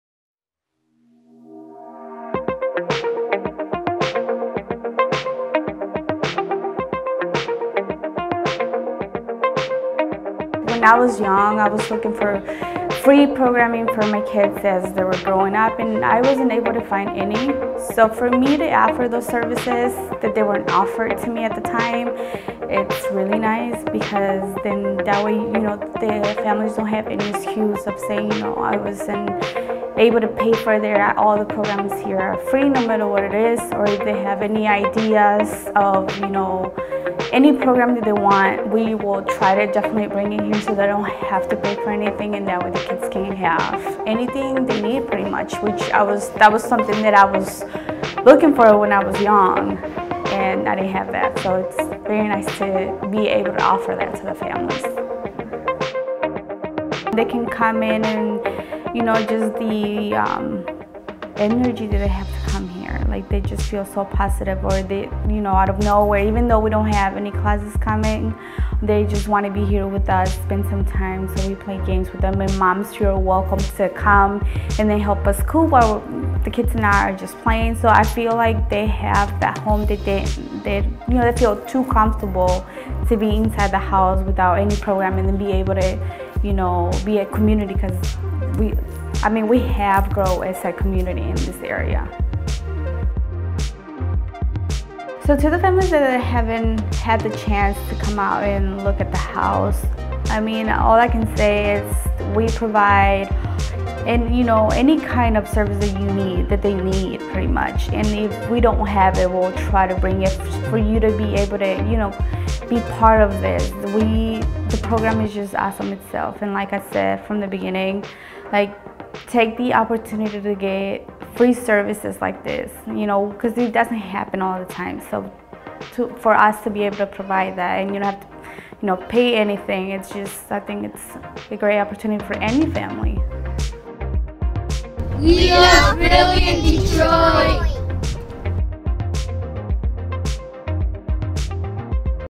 Brilliant Detroit Springwells
Learn more about our Springwells hub, where we have free education, health, and family support programming for kids as they’re growing up. If we don’t have a program you want, let us know and we’ll try to find it! Of course, kids show up even when there aren’t programs because of the community here. It’s a great opportunity for any family!